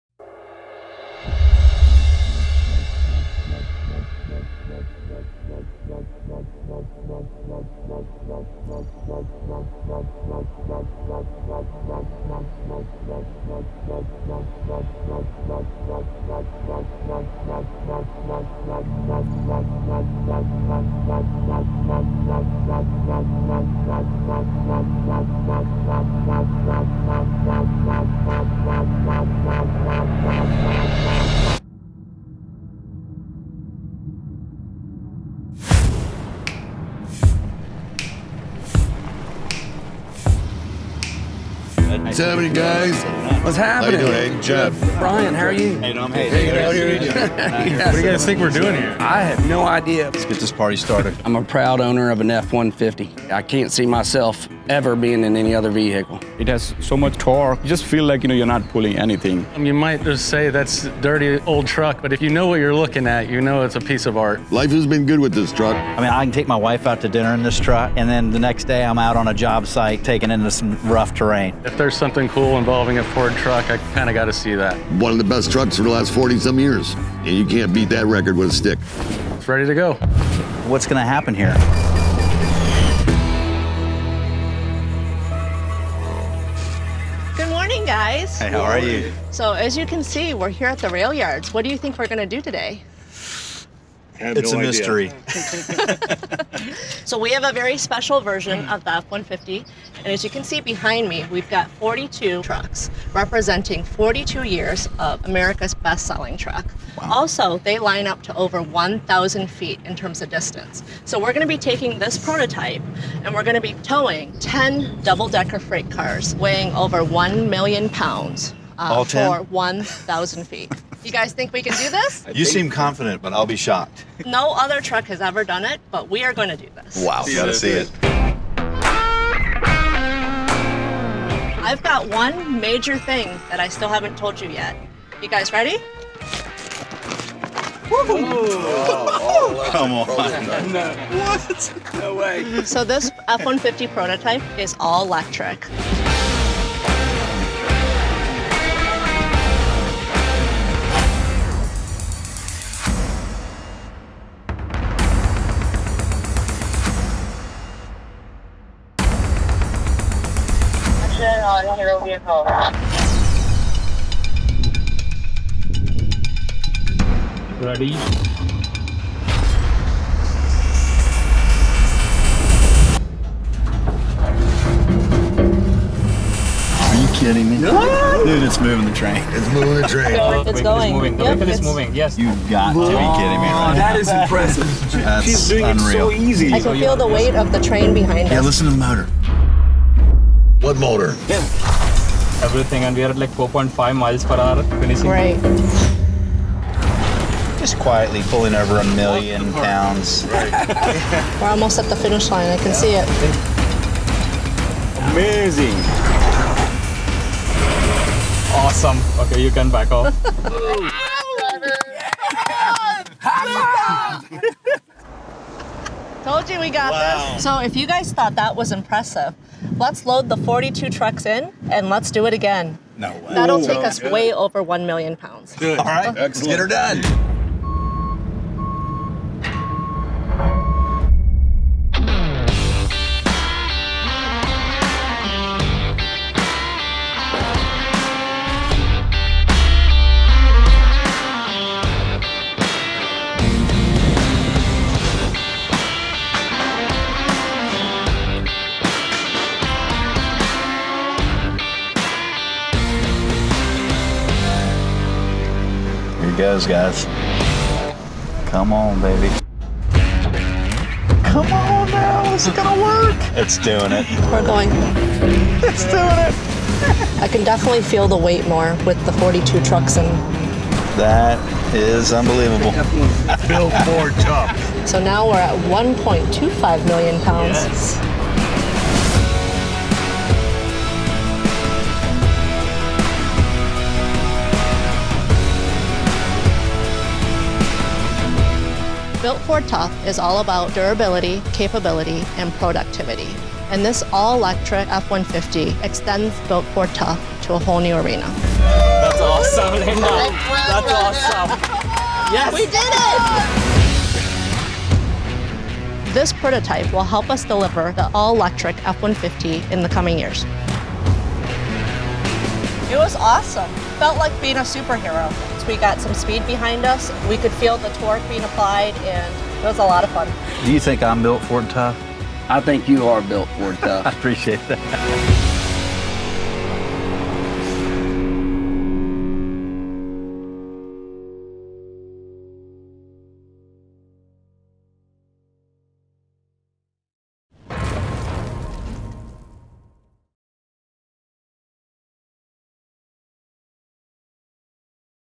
All-Electric F-150 Prototype: Tows 1M+ Pounds | Ford
We’ve confirmed we are bringing an all-electric F-150 to market. Now, we’re showing you the capability you asked for by having it tow more than 1 million pounds.* This is Built Ford Tough.

*The F-150 battery-electric prototype is towing far beyond any production truck’s published capacity in a one-time short event demonstration. Never tow beyond a vehicle’s towing capacities. Always consult the Owner’s Manual.